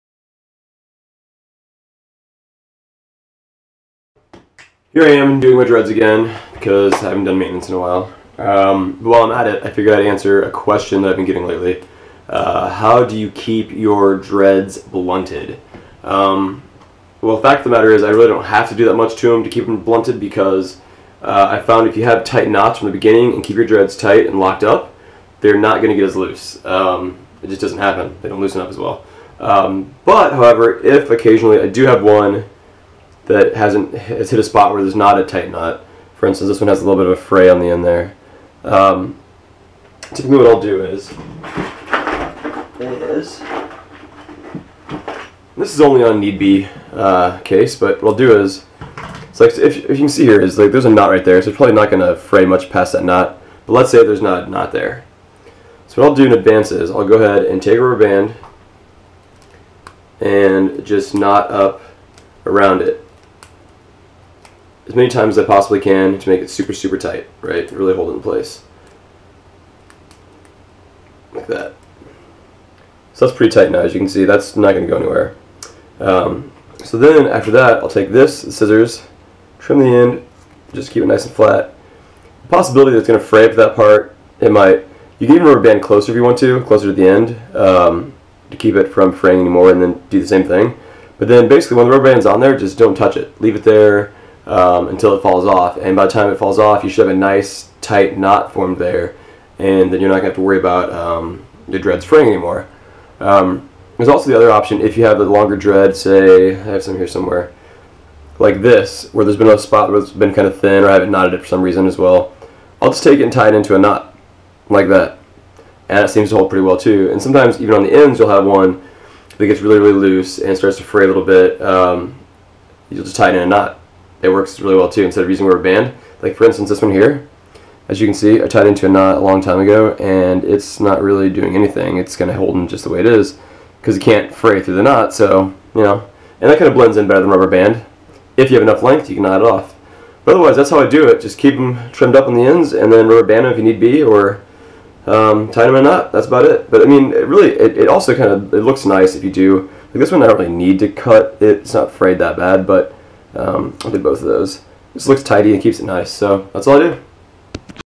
How to round (blunt) the tips of your dreadlocks
Have you ever wondered what it takes to start dreadlocks? Thought to yourself how do you create dreadlocks? Well, let me break down the ins and out of dreads for you. I’ve had my dreadlocks for two decades so I’ve experienced the good, the bad, and everything in between.

In this video I explain the easiest way to round out the tips of your dreadlocks.

Shot on a GoPro